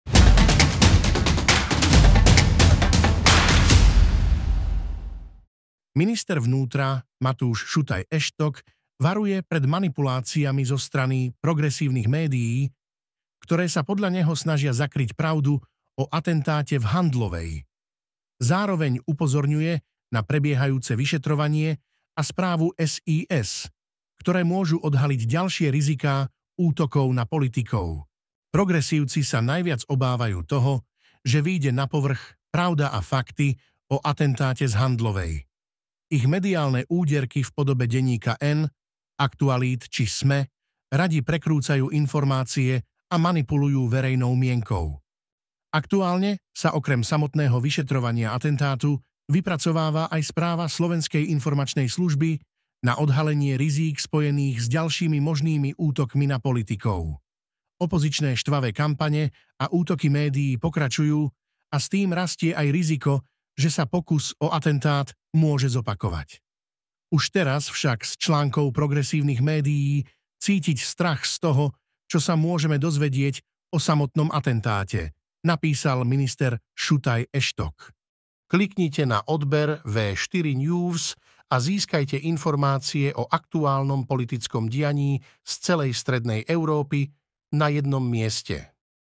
MINISTER EŠTOK: PROGRESÍVCI SA OBÁVAJÚ, ŽE PRAVDA O ATENTÁTE VYJDE NA POVRCH!
Minister vnútra Matúš Šutaj Eštok varuje pred manipuláciami zo strany progresívnych médií, ktoré sa podľa neho snažia zakryť pravdu o atentáte v Handlovej. Zároveň upozorňuje na prebiehajúce vyšetrovanie a správu SIS, ktoré môžu odhaliť ďalšie riziká útokov na politikov.